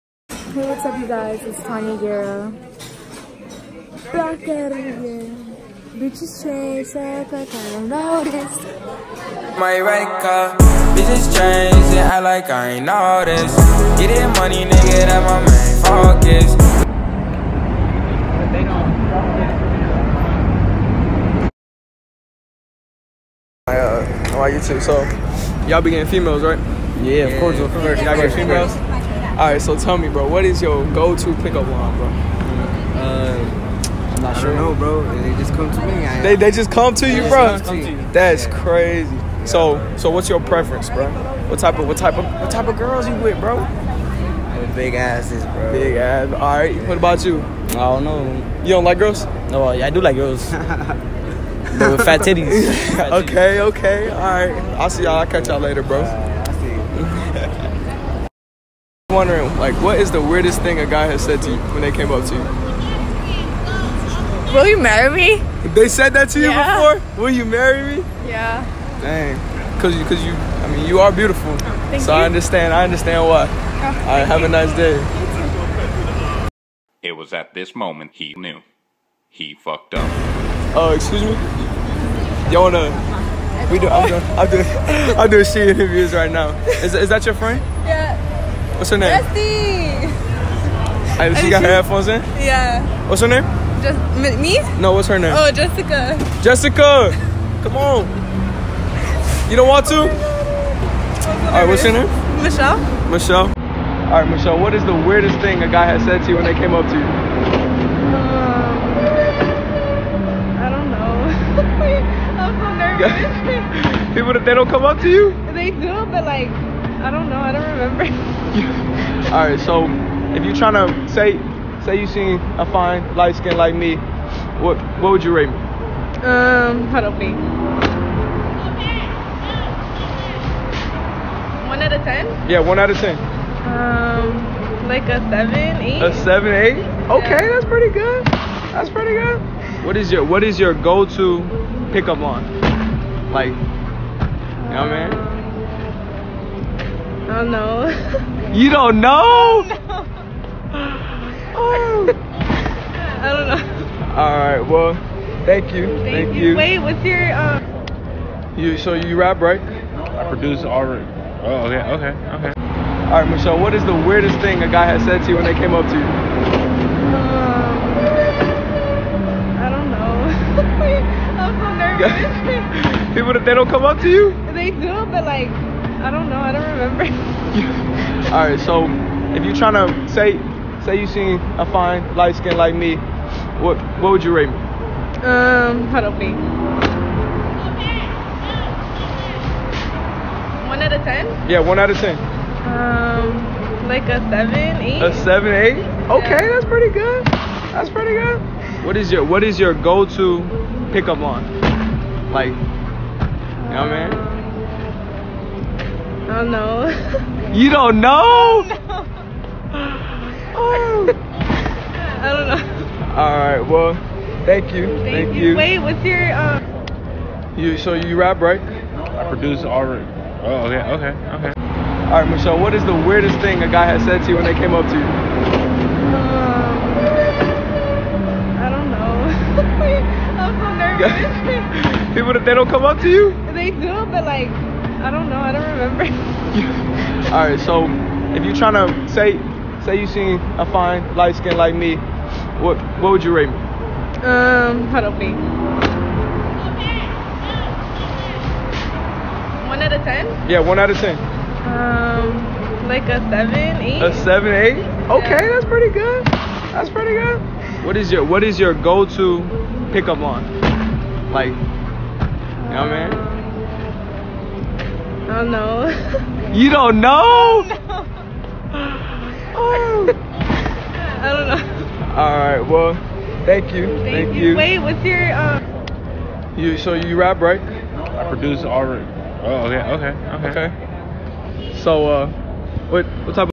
Street interviews on Hollywood blvd
A lot of them was scammers 😂😂